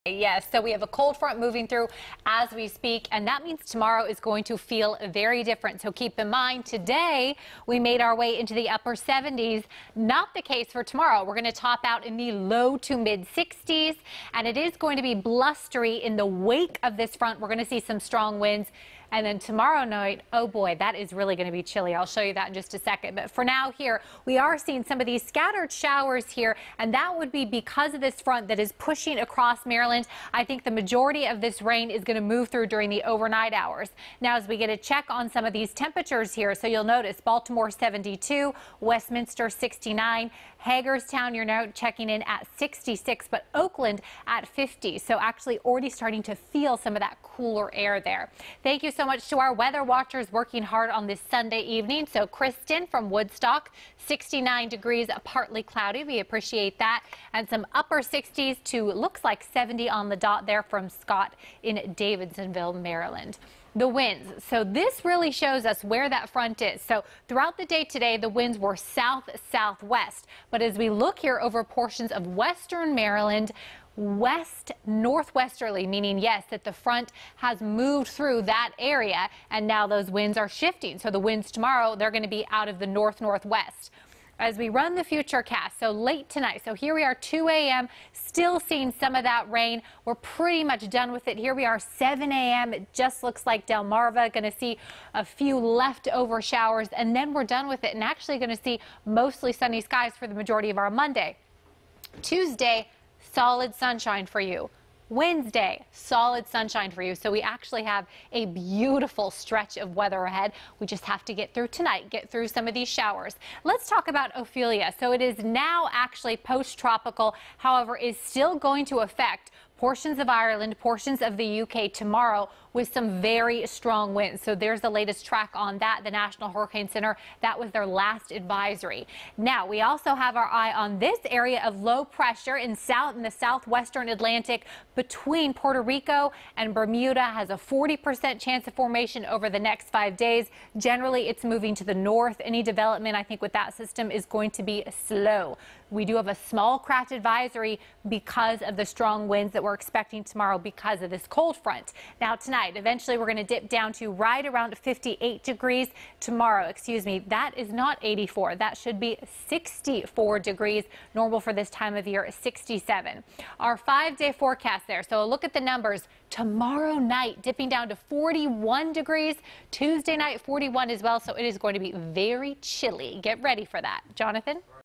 Meg McNamara Has Your Overnight Weather Forecast
Meg McNamara has your Sunday overnight forecast.